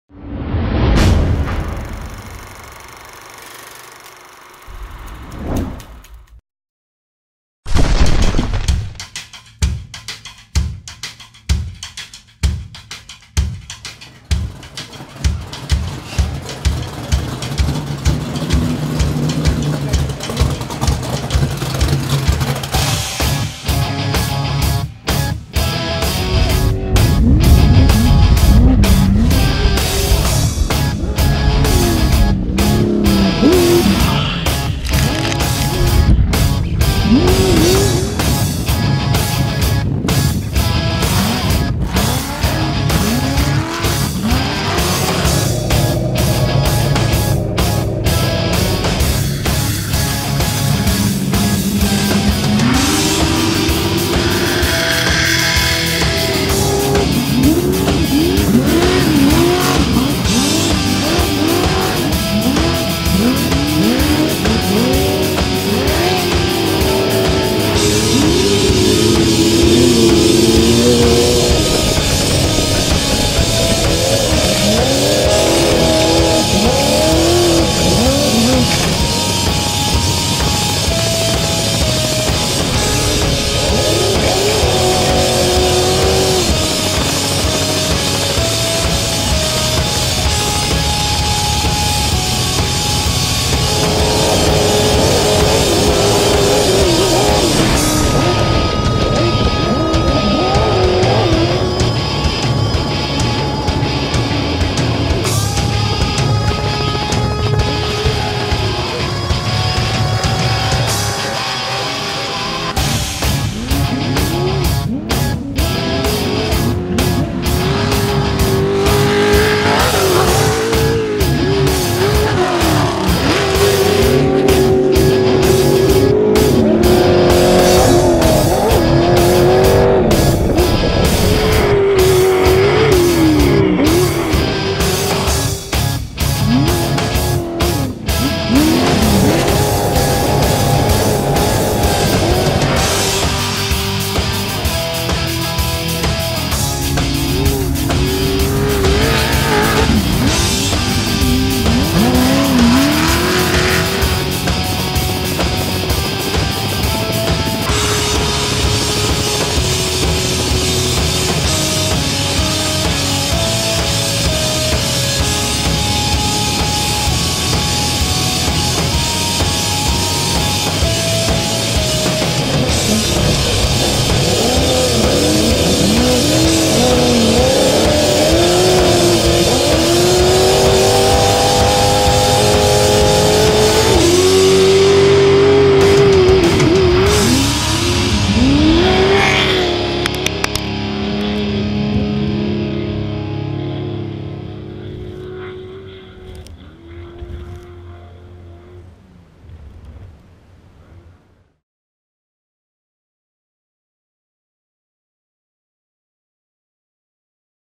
Concrete Motorsports 2016 Laughlin Desert Classic
Highlights of #1590 Concrete Motorsports during the 2016 Laughlin Desert Classic.